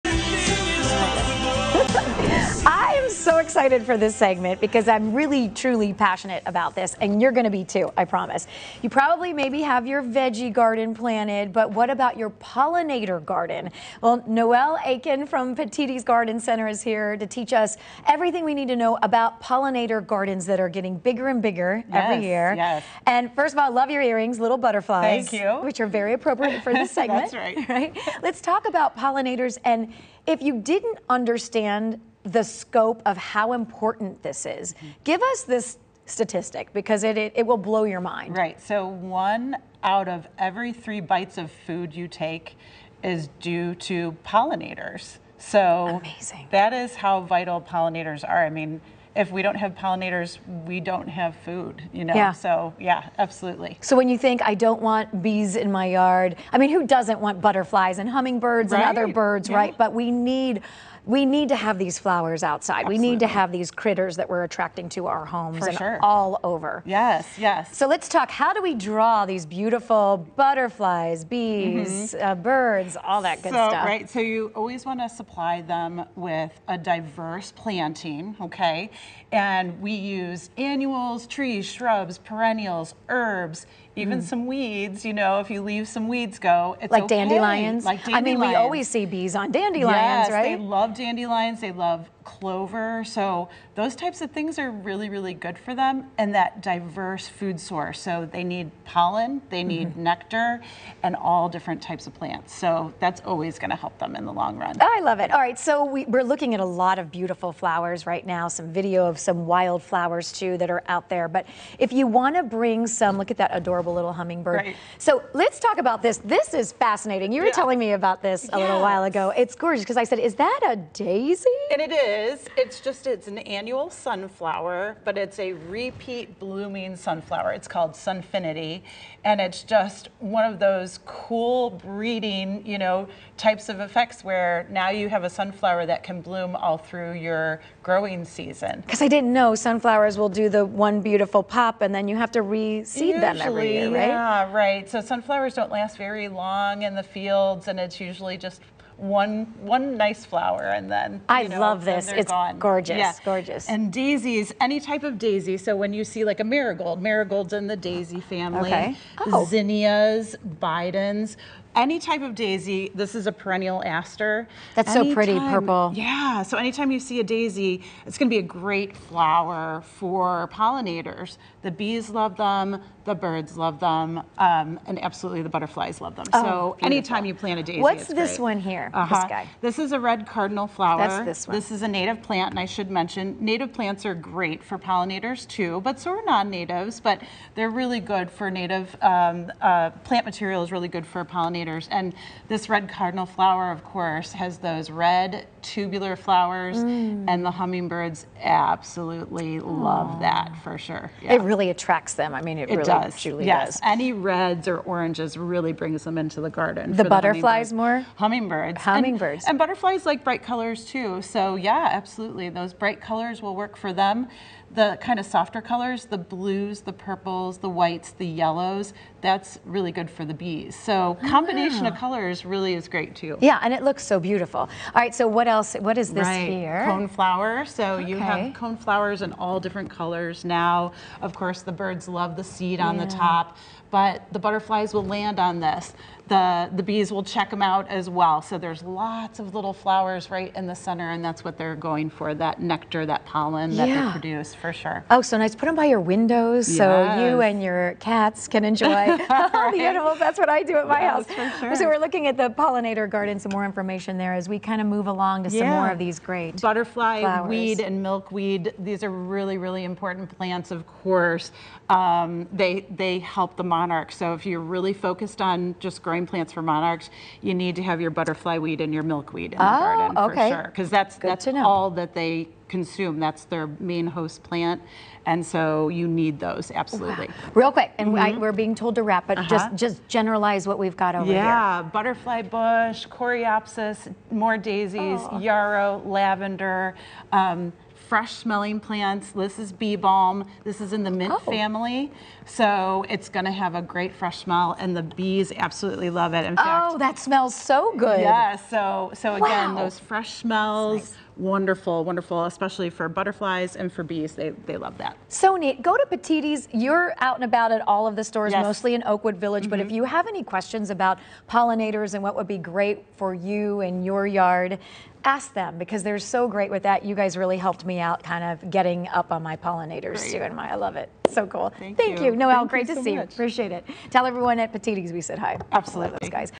Pollinator Gardening tips from Petitti Garden Centers
Did you realize that one out of every three bites of food we consume is thanks to pollinators? Fox 8's Stefani Schaefer learns the basics about pollinator gardening in Northeast Ohio from Noelle Akin, Director of Education for Petitti Garden Centers.